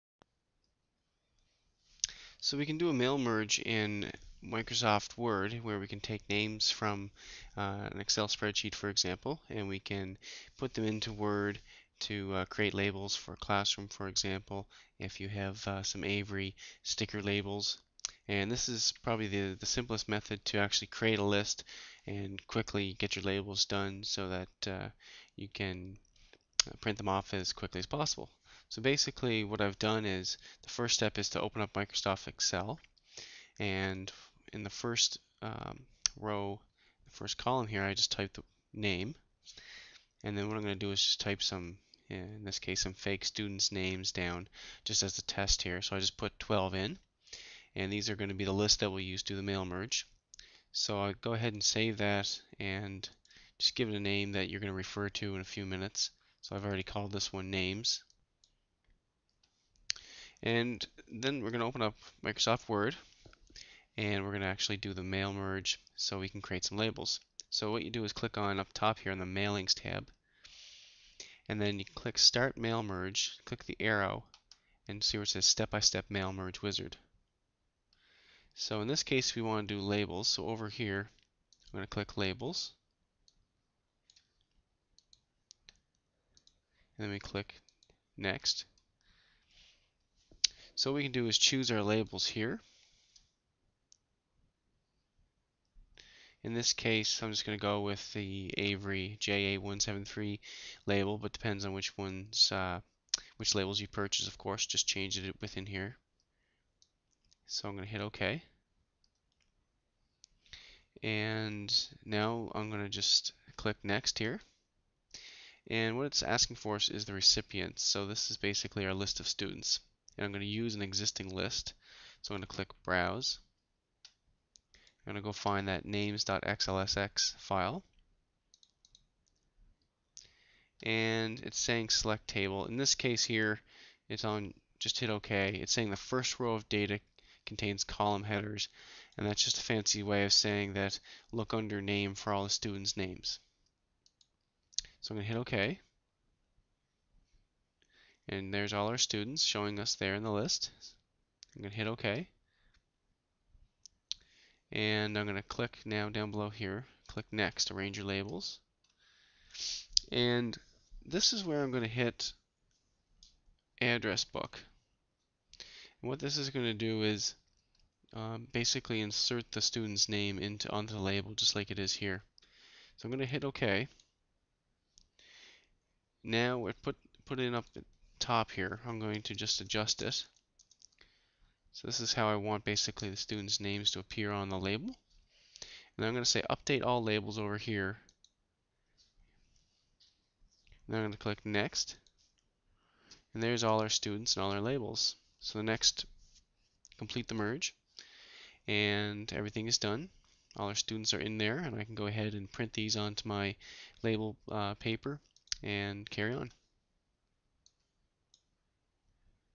Word 2007 Labels mail merge -- to create class labels for students
Use Excel and Word to quickly create a list of students and sticky labels for classroom uses.